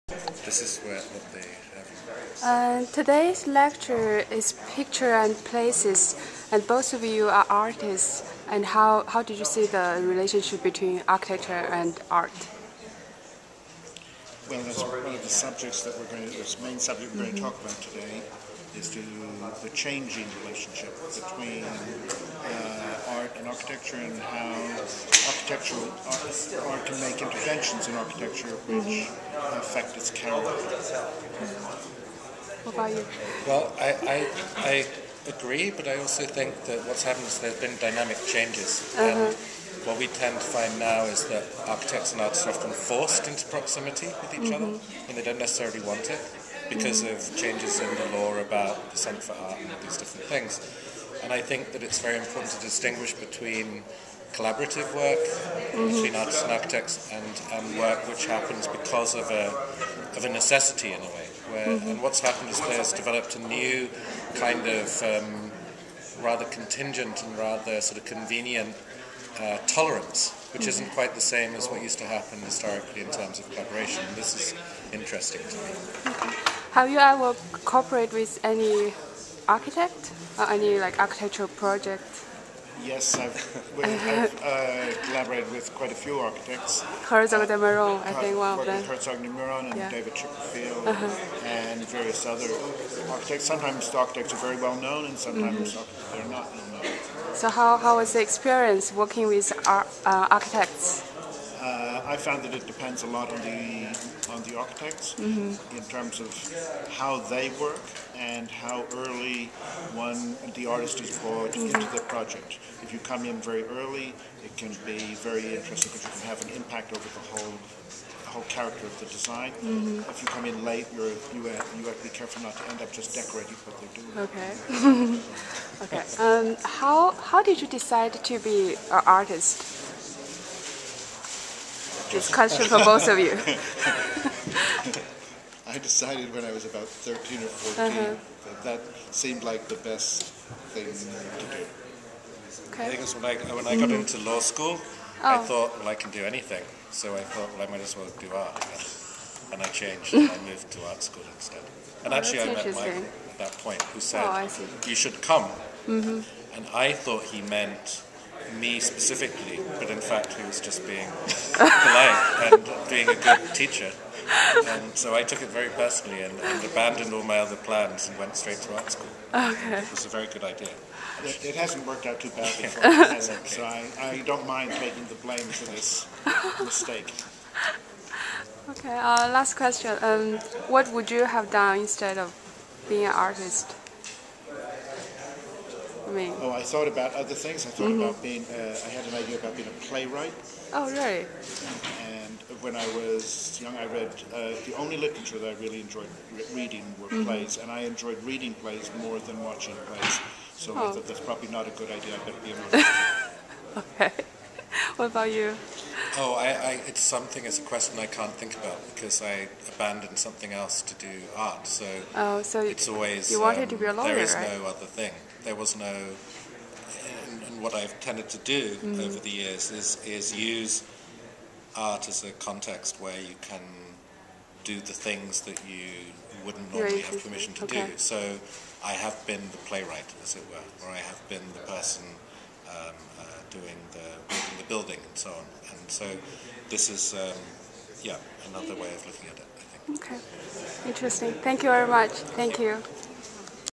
An Interview with artists Michael Craig-Martin and Liam Gillic
An Interview with artists Michael Craig-Martin and Liam Gillick,  during which they discussed the relationship between art and architecture, and involvement in architectural project as artists. The interview was conducted on February 11, 2010 by 1st Year CCCP student Tong Tong in  Avery Hall.